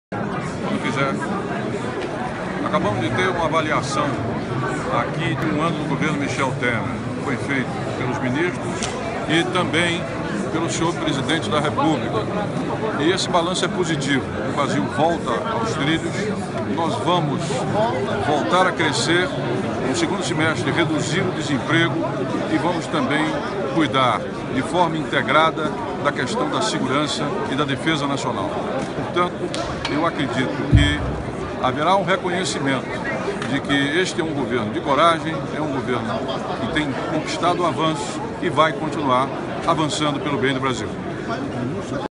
Um ano de governo
Ministro Raul Jungmann destaca que os avanços obtidos pelo governo federal nos setores de segurança pública e defesa nacional nos últimos 12 meses vão ser mantidos de forma integrada.